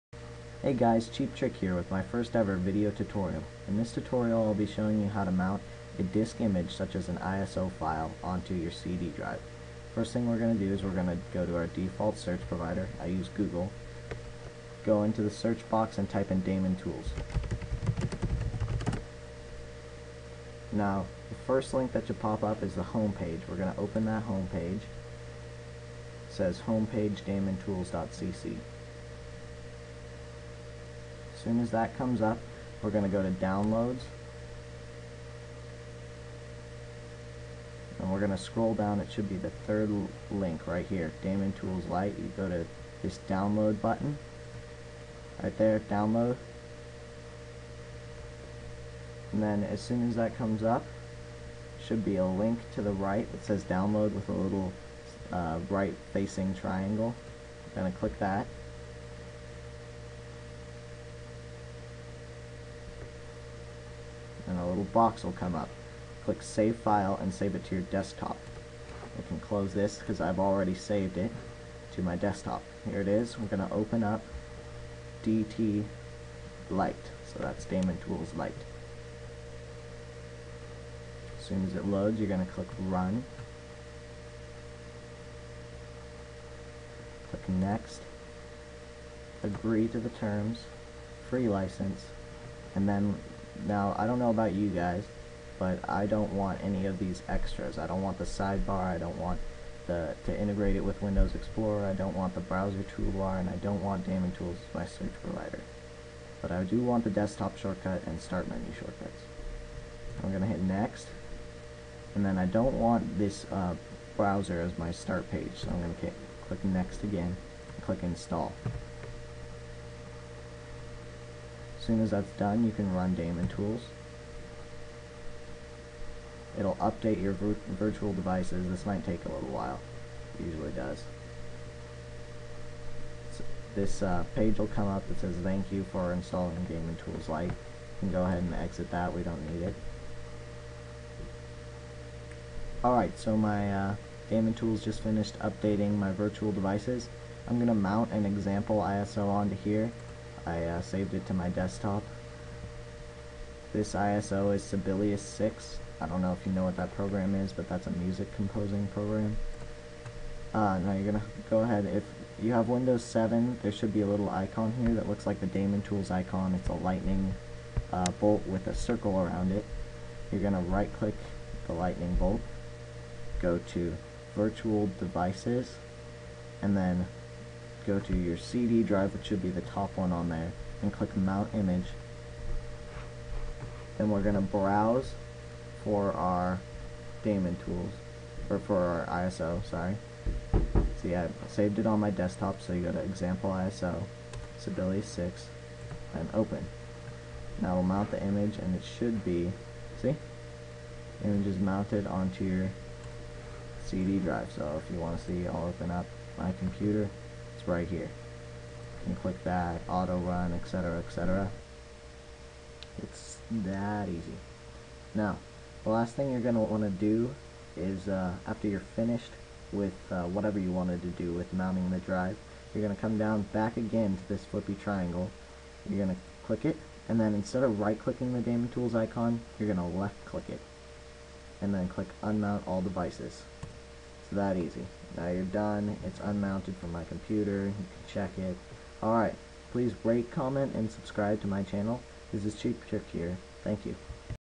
How to Mount a Disc Image to a CD Drive (Windows Tutorial)
In this video I will show you (the viewer) how to mount a disc image to a CD drive.

Daemon Tools Home Page...